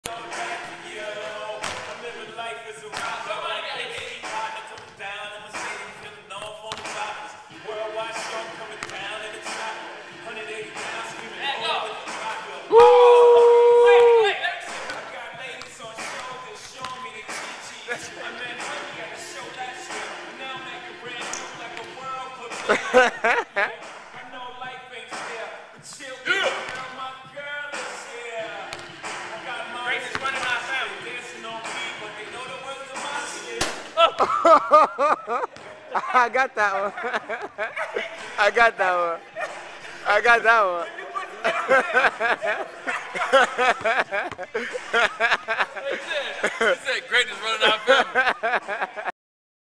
Dwyane Wade sinks a half court shot
Dwade swishes a half court shot and dances afterwards
his friend tries and completely bricks it.